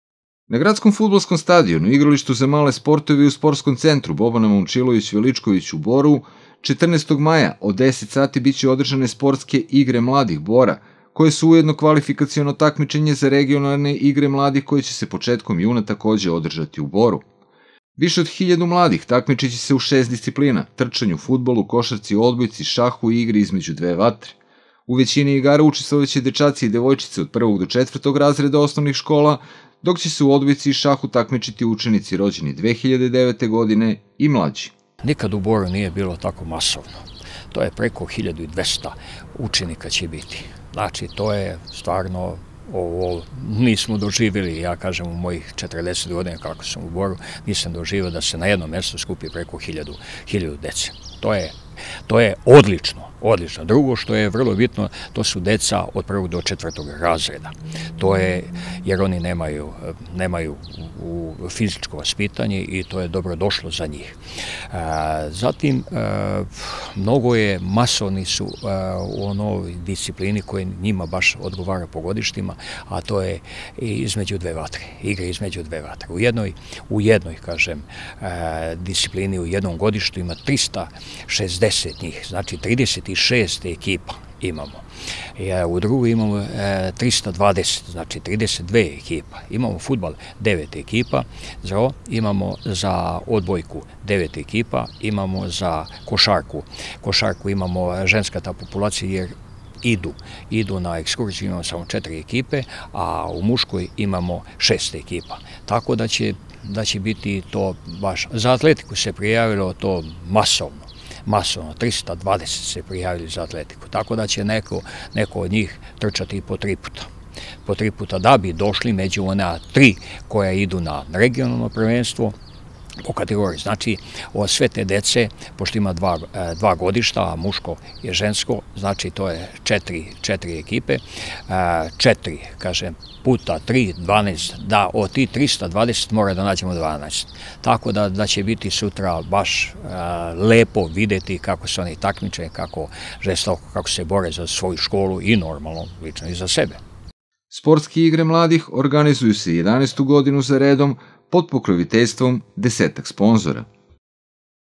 „Sportske igre mladih“ 14. maja u Boru", 13. maj 2024. (RTV Bor)
Bor će i ove godine učestvovati u Sportskim igrama mladih, koje se već 11 godina organizuju u većini gradova u Srbiji. Učenici osnovnih škola takmičiće se u šest disciplina, a najbolji će se plasirati na regionalno takmičenja koje će se takođe održati u Boru.